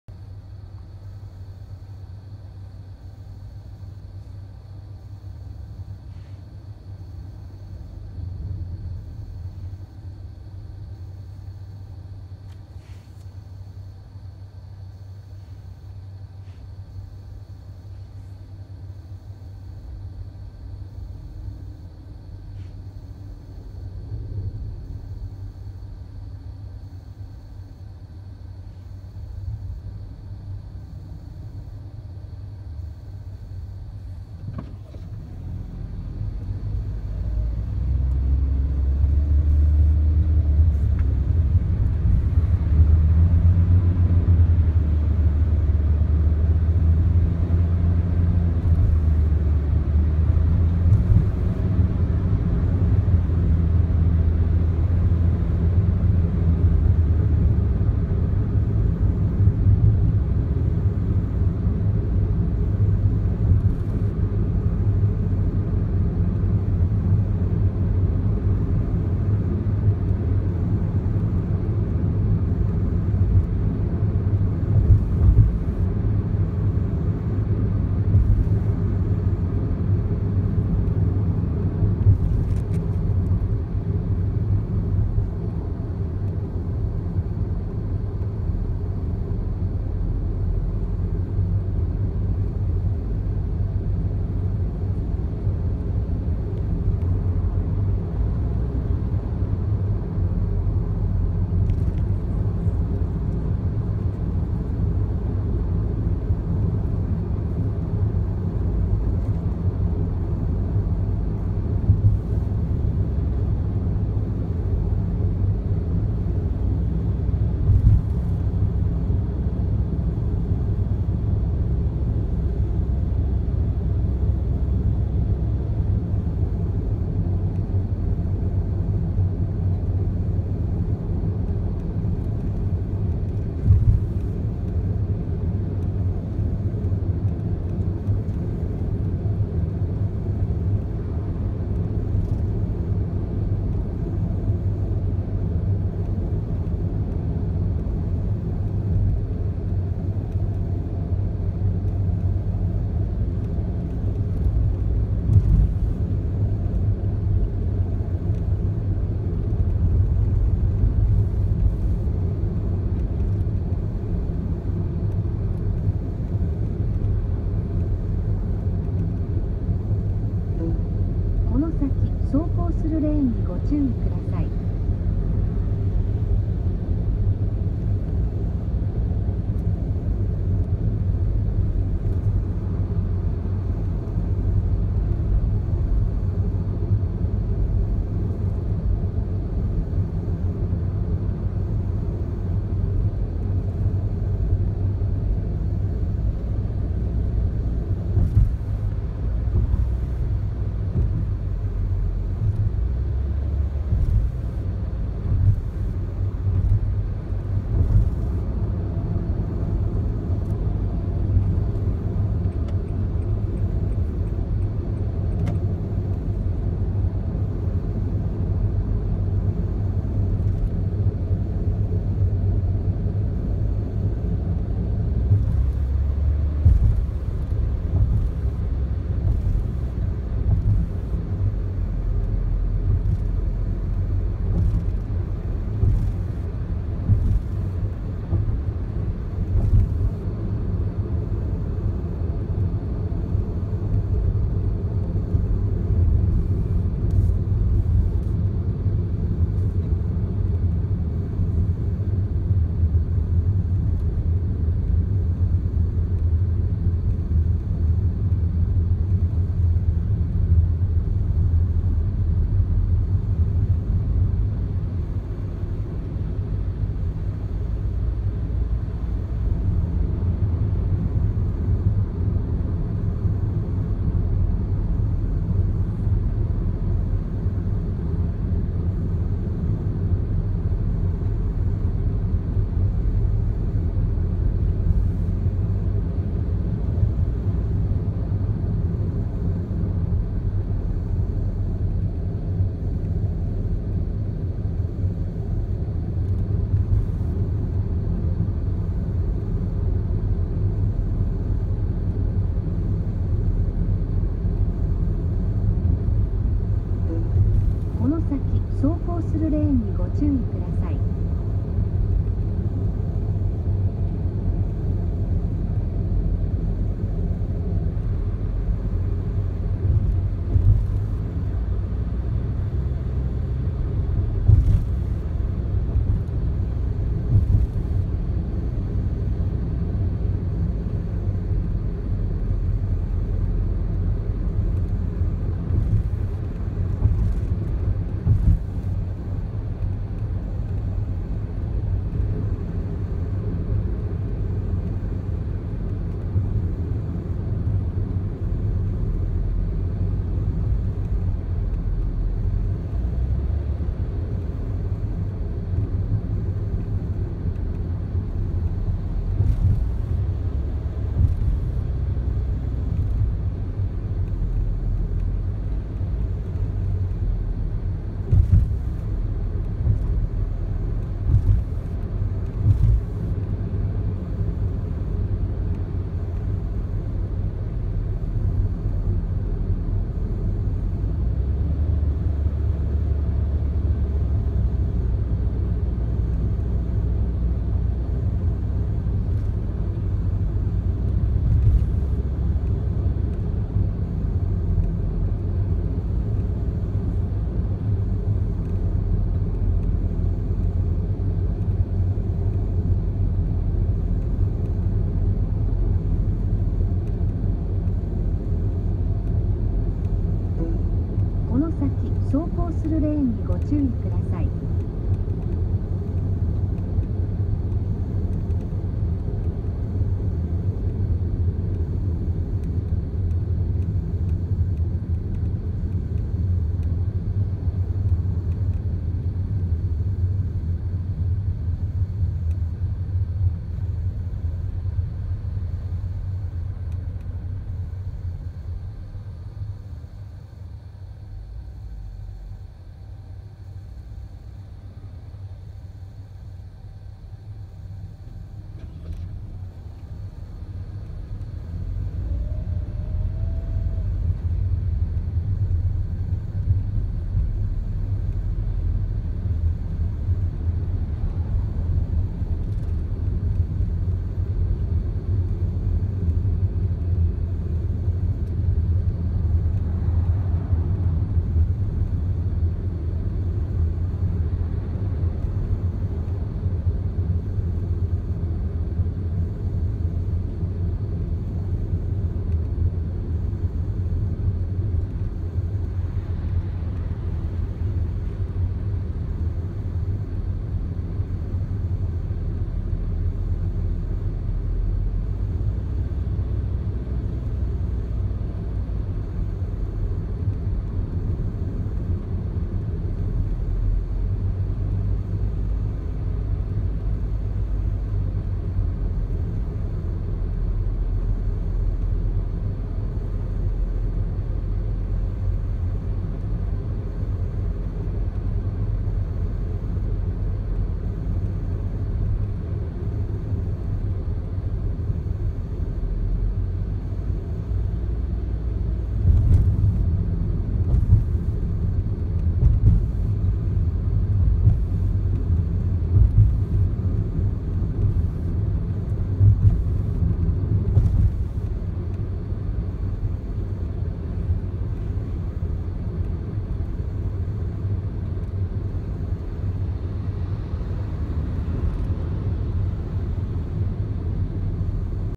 【試乗】HONDA N-BOX Custom G・Lパッケージ FF CVT
代車で乗っているエヌボックス、走行時を撮影しました。